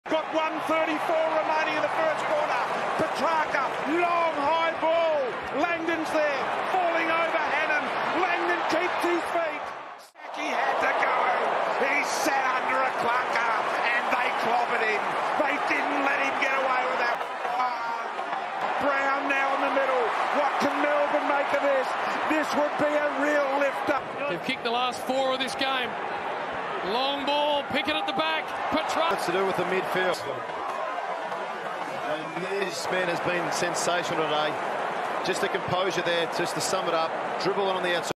Kysaiah Pickett's little actions in the Grand Final
Credit to user r/The_Frig on reddit for the vision. I'm using it as an example in an article on themongrelpunt.com